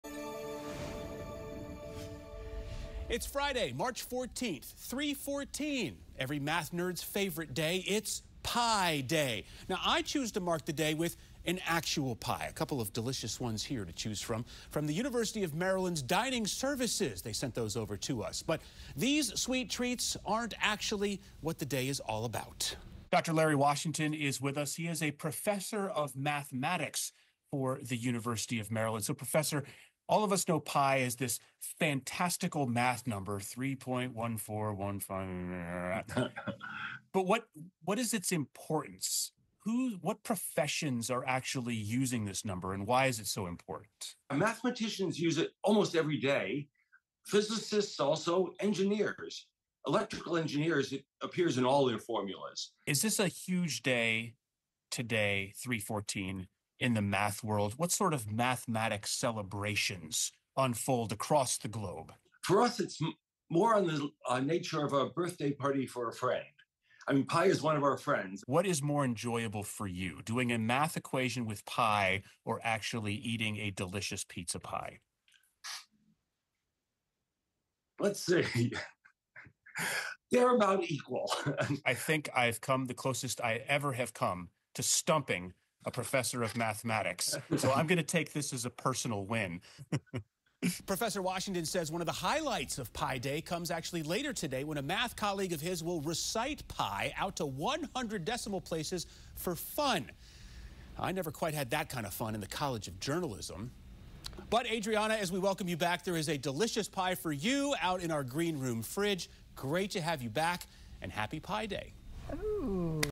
Happy Pi Day! March 14 or 3/14, the day that every math nerd eagerly anticipates each year
On world  "Pi Day," 7News is taking the time to celebrate one of mathematics' most famous constants: Pi, the ratio of a circle's circumference to its diameter, approximately 3.1415.

While some people may opt for a slice of pie to mark the occasion, others take a deeper dive into the fascinating mathematical significance of this number. We spoke to a mathematician about it's significance.